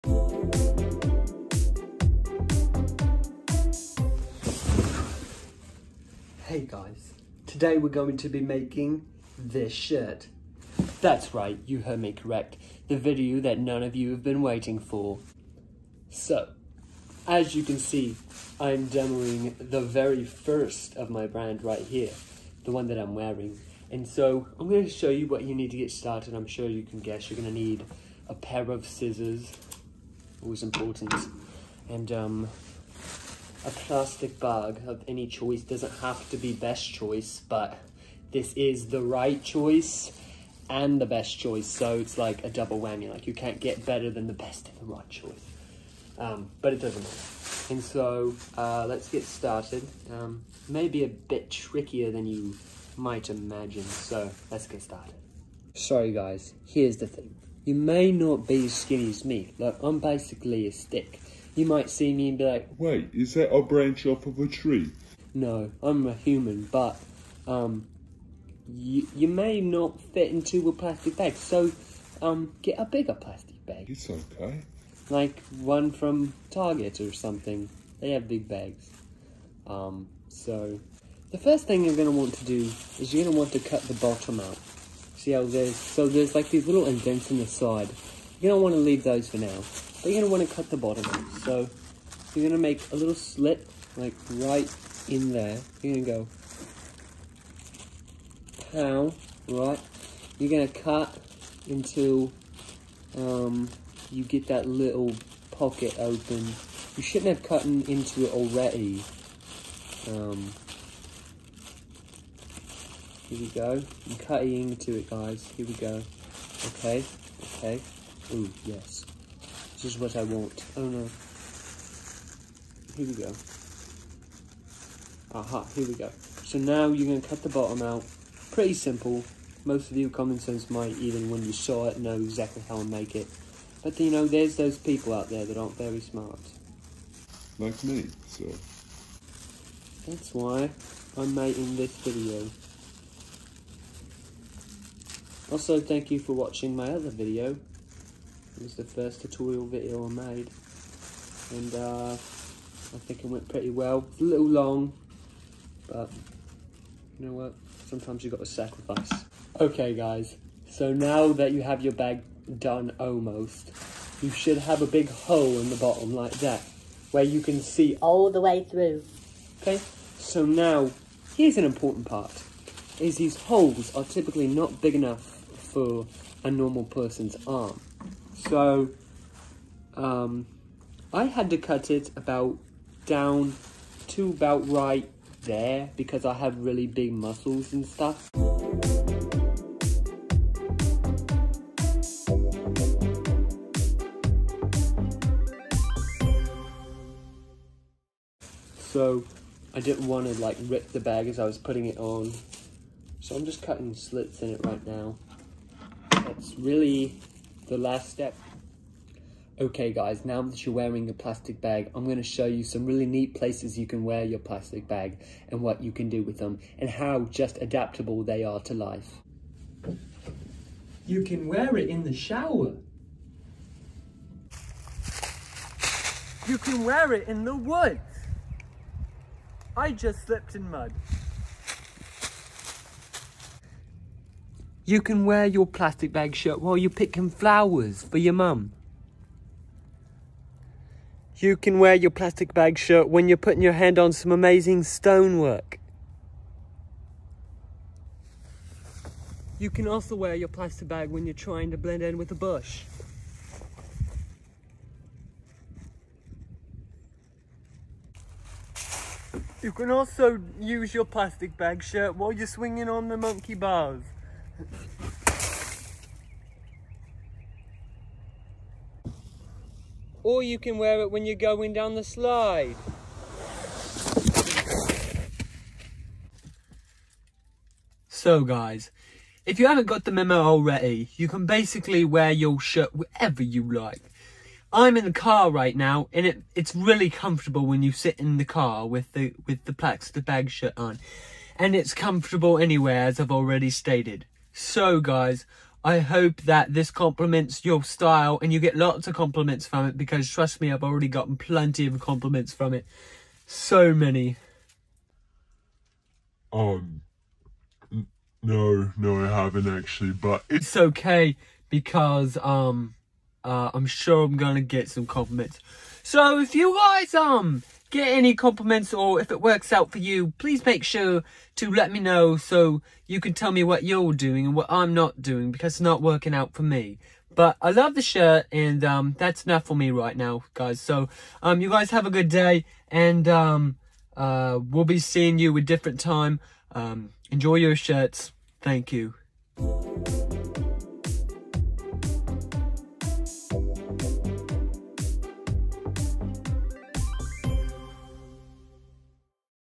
DIY Plastic Bag Shirt
I promised I'd show you how to make my plastic bag shirt so here it is.  I've gotten a lot of complements on it, well, actually I haven't. Enjoy!